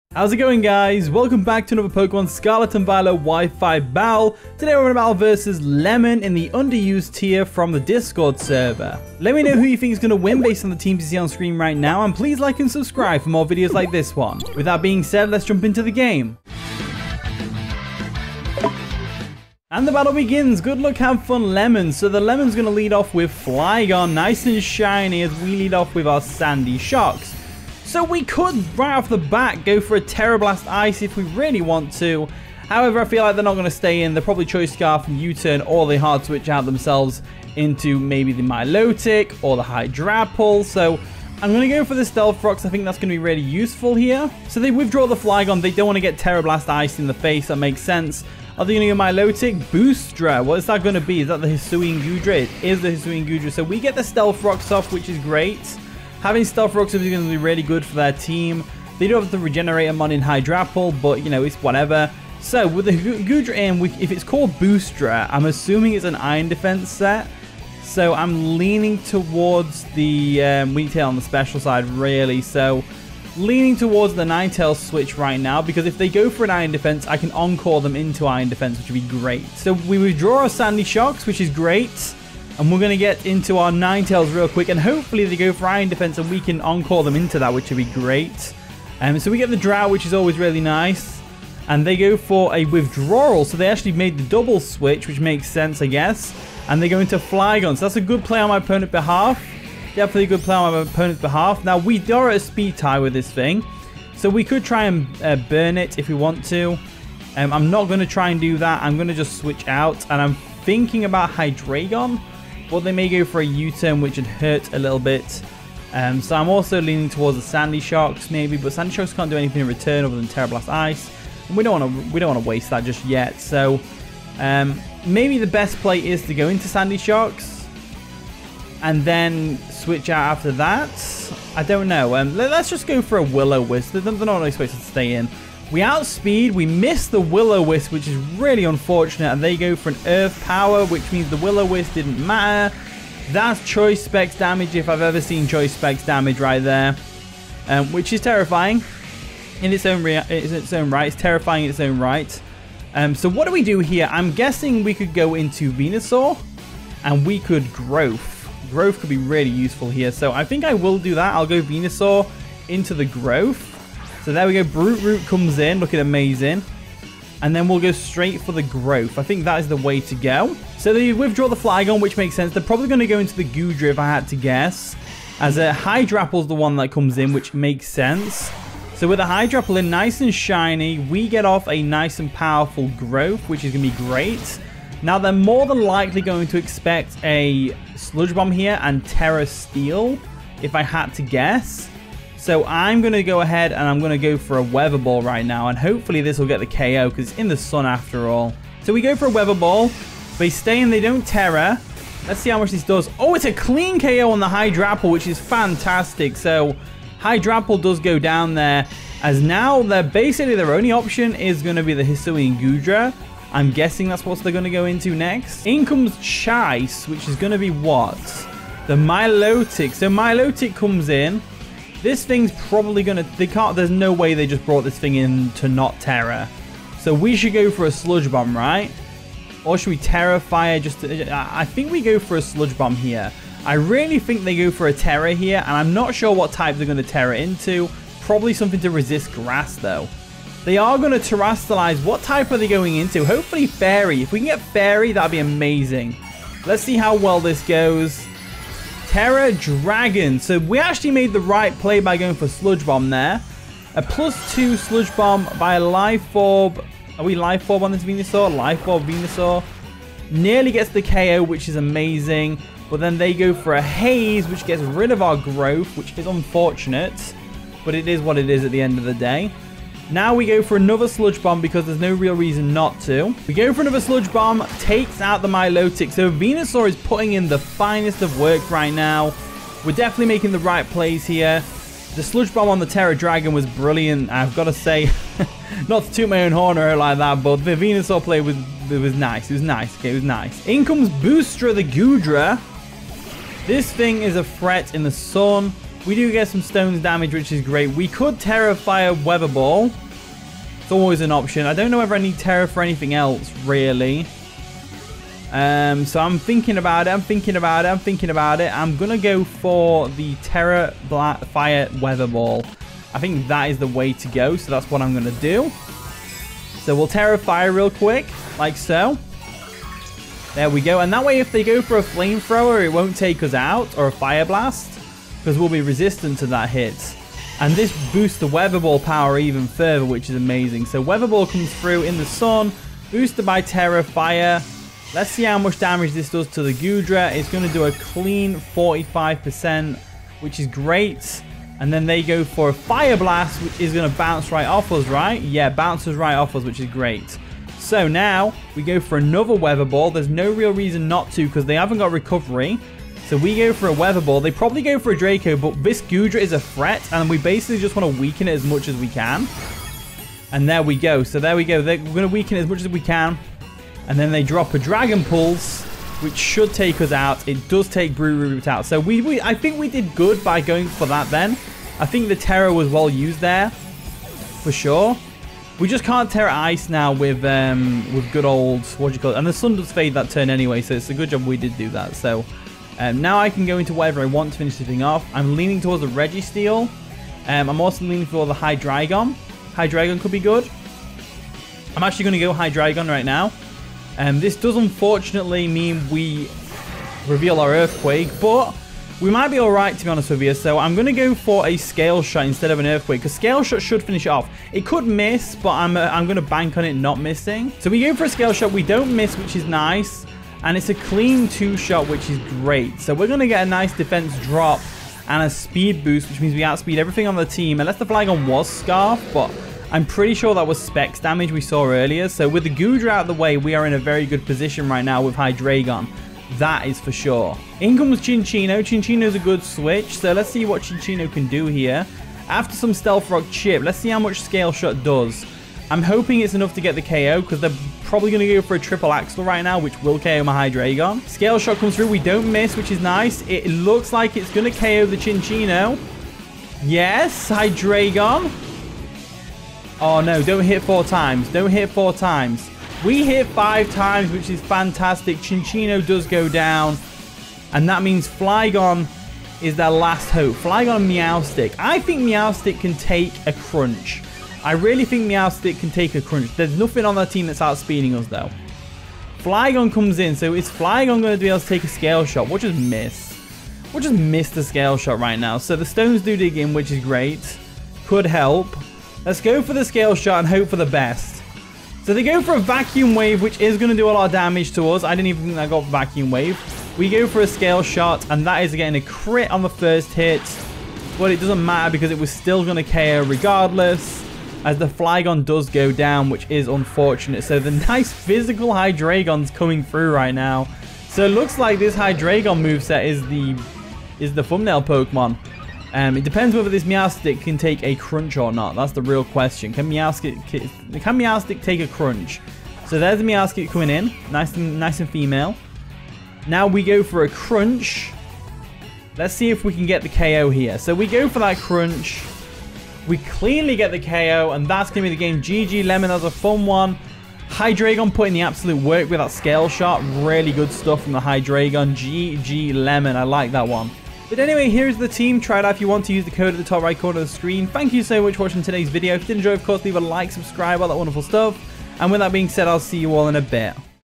This ONE MOVE Makes Hydreigon BROKEN!
Today's battle we utilise a very unique Hydreigon moveset which goes off hard!